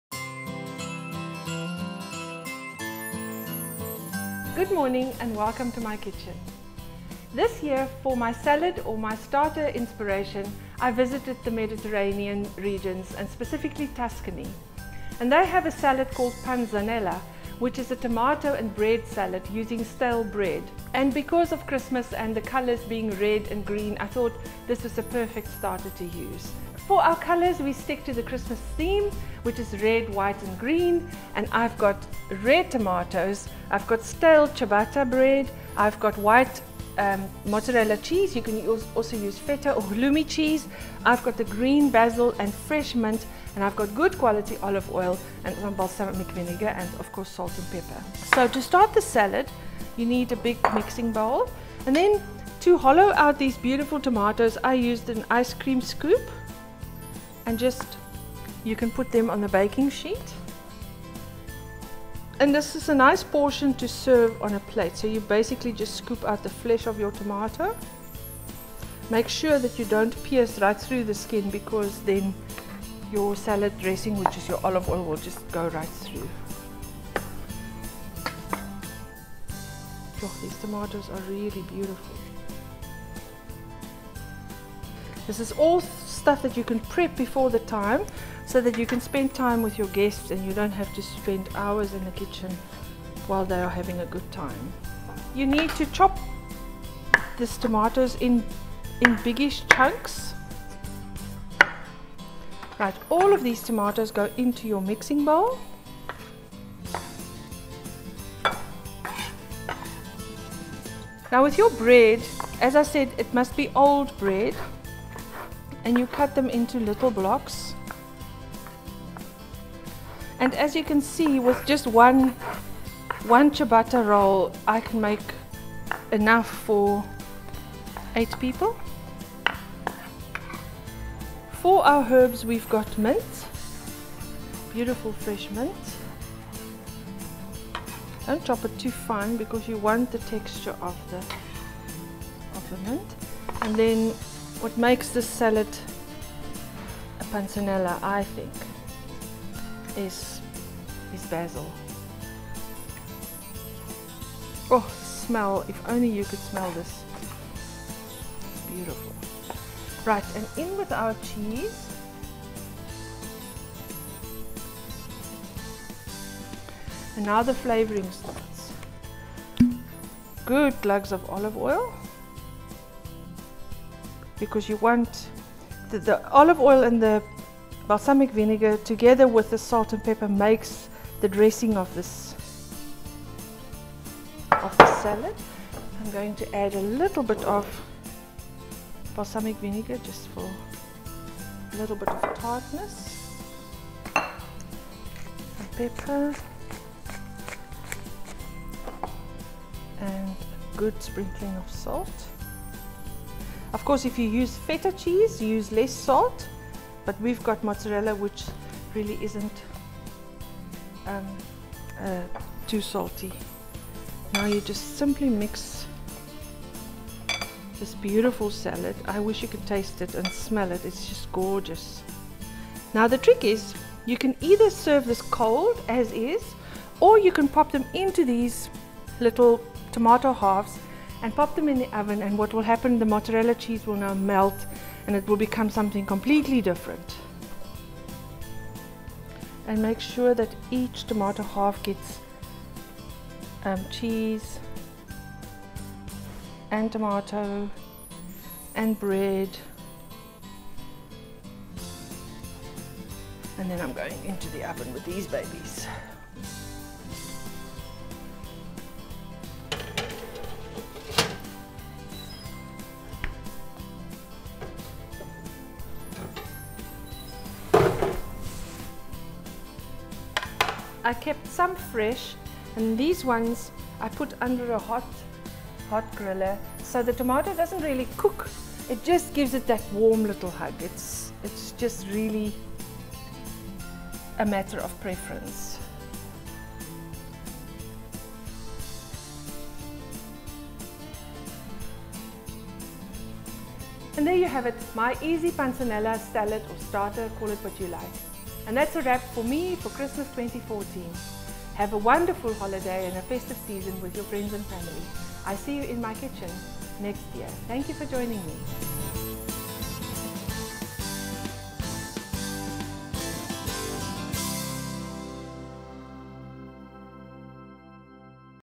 My Easy Cooking - Stuffed Tomatoes
Nina Timm from My-easy-cooking.com shares her delicious stuffed tomatoes recipe for the festive season. Enjoy !

Mumspiration.com